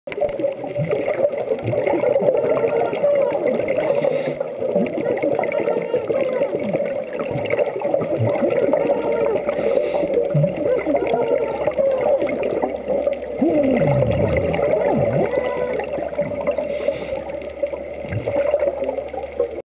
Stingray Covered 11 8 14
This Ray covered himself except for his eyes. We were in only 25 feet of water